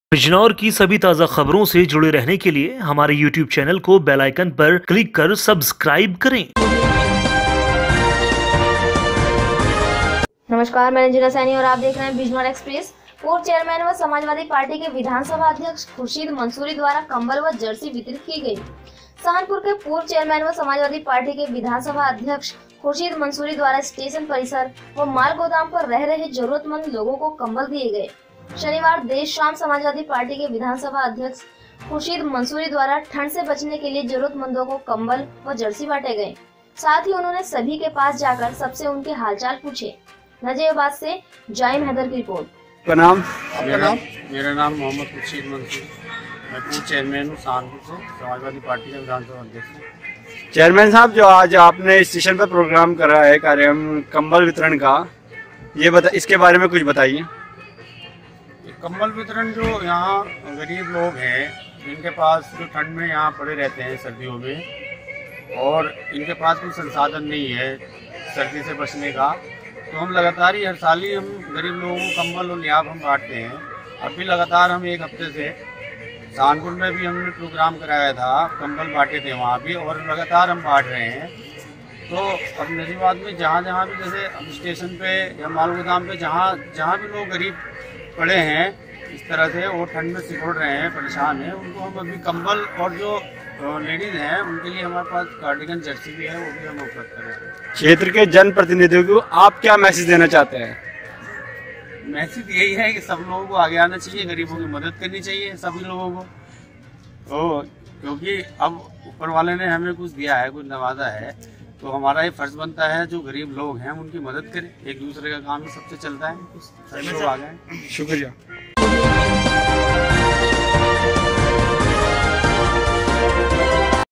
पूर्व चेयरमैन व समाजवादी पार्टी के विधानसभा अध्यक्ष खुशीँद मंसूरी द्वारा कंबल व जर्सी वितरित की गई
साहनपुर के पूर्व चेयरमैन व समाजवादी पार्टी के विधानसभा अध्यक्ष खुशीँद मंसूरी द्वारा स्टेशन परिसर व माल गोदाम पर रह रहे जरूरतमंदों लोगों को कंबल दिये गये।
शनिवार देर शाम समाजवादी पाटीँ के विधानसभा अध्यक्ष खुर्शीद मंसूरी द्वारा ठंड से बचाने के लिए जरूरतमंदो को कंबल व जर्सी  बांट गये। हैं।  साथ ही उन्होंने सभी के पास जाकर सबसे उनके हाल चाल पूछा।

नजीबाबाद से ज़ाईम हैदर की रिपोर्ट

Download Bijnor Express Android App from Play Store:
shorturl.at/juAEL